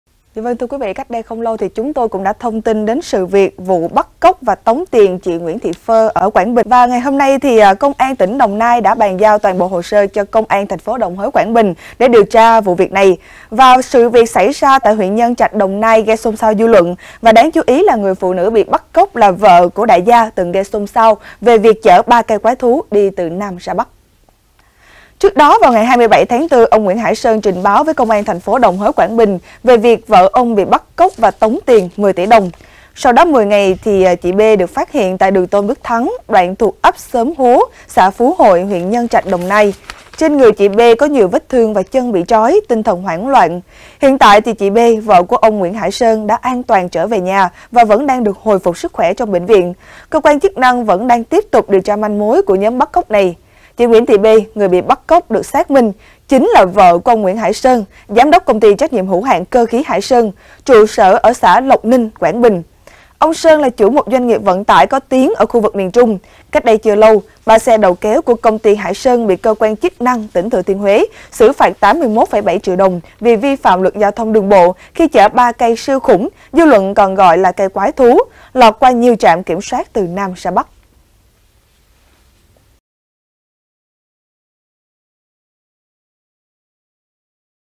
Điều ít biết về đại gia có vợ bị bắt cóc | VTC9
VTC9 | Chồng của người phụ nữ nghi bị bắt cóc đòi 10 tỉ đồng tiền chuộc là một "ông trùm" vận tải ở Quảng Bình, người vốn không xa lạ với cộng đồng mạng thời gian qua.